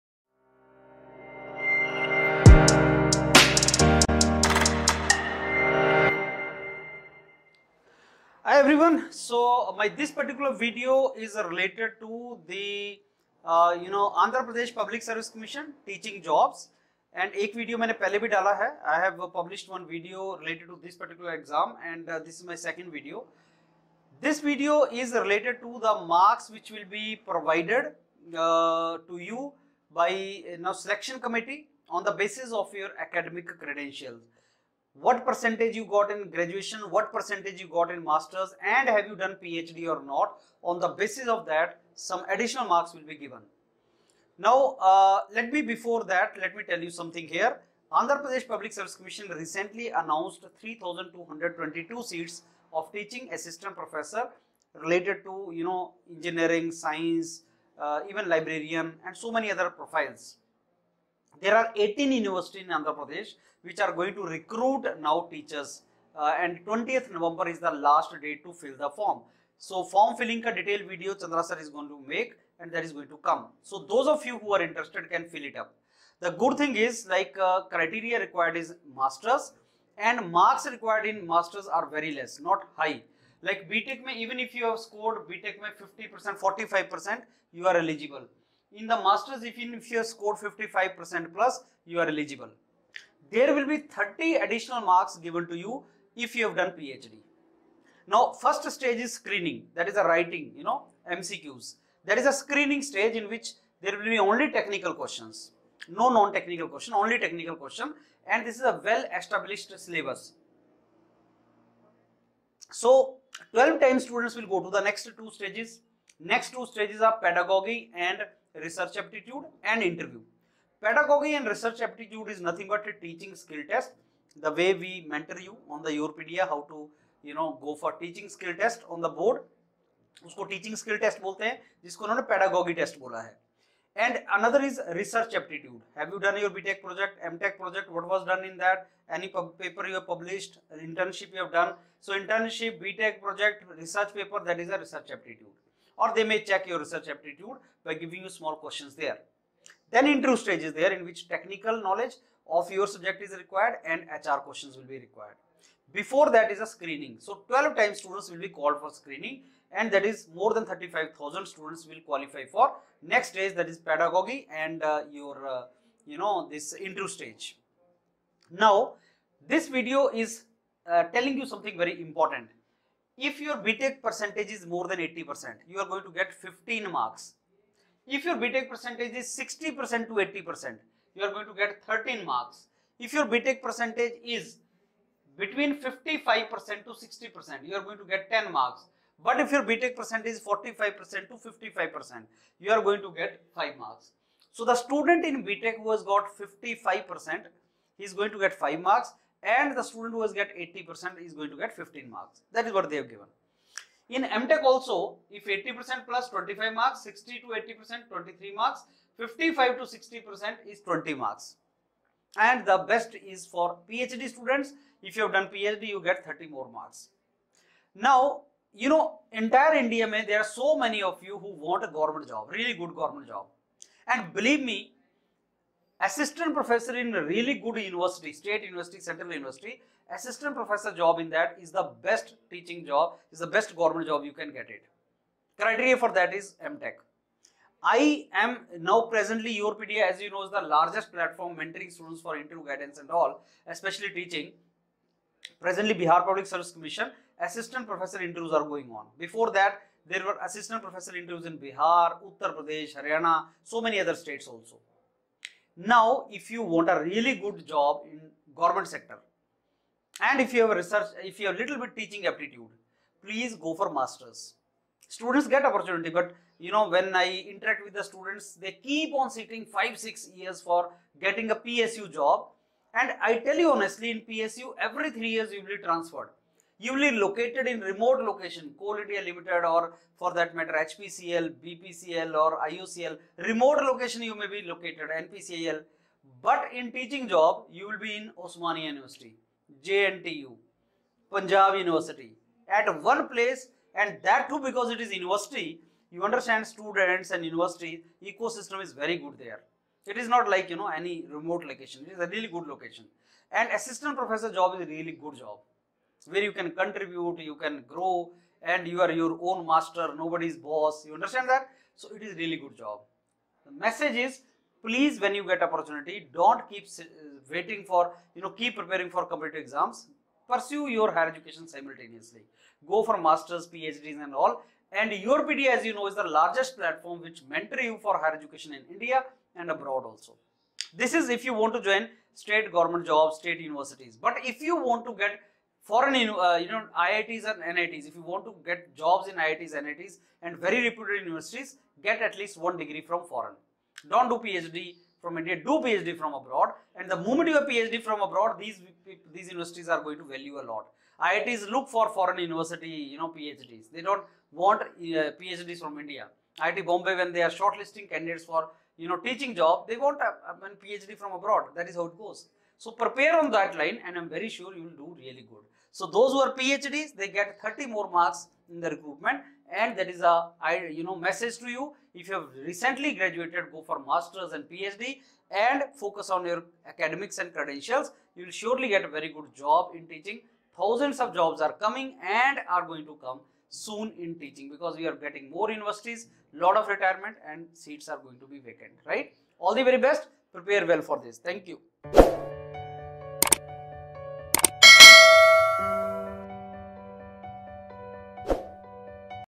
30 Additional marks for PhD | 3200+ Vacancies as Assistant professor in all Disciplines
3200+ Vacancies as Assistant professor in all Disciplines | RGUKT AP Recruitment Notification 2023

Fill the Google form for APPSC AP written Test & Interview